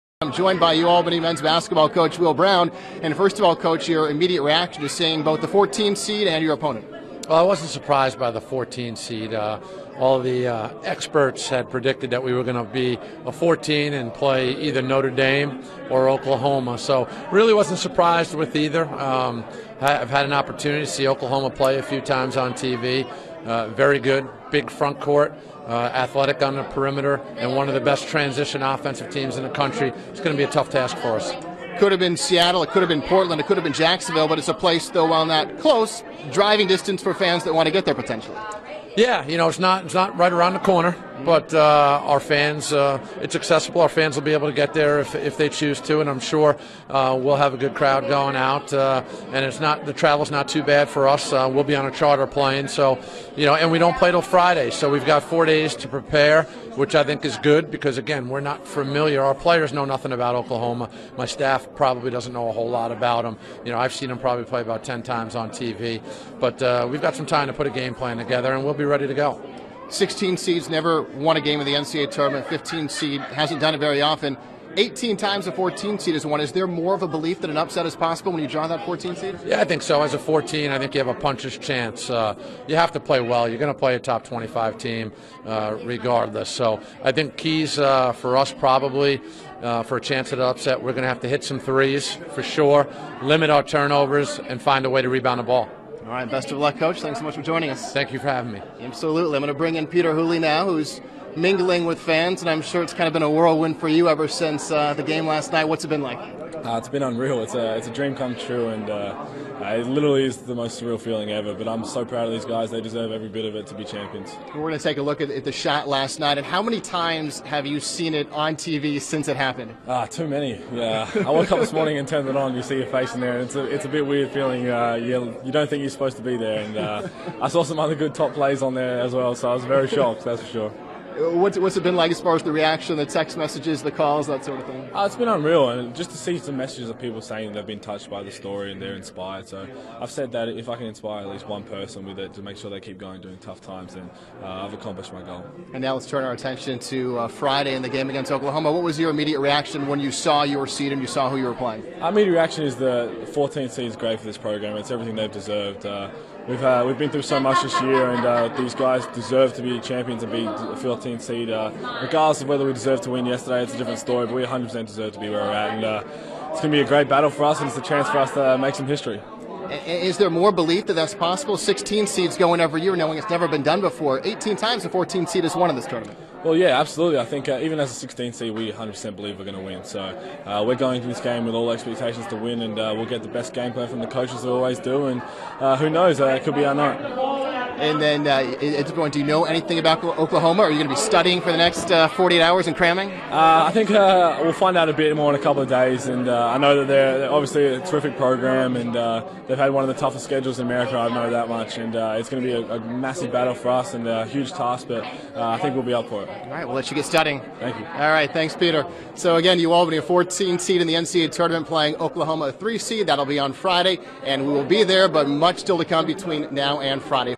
UAlbany Basketball talk Oklahoma
The UAlbany men's basketball team earned a 14 seed in the NCAA tournament. The Great Danes will play Oklahoma on Friday in Columbus, OH. Coach Will Brown and captain Peter Hooley share their initial thoughts on their seed and their opponent from the selection show party on campus.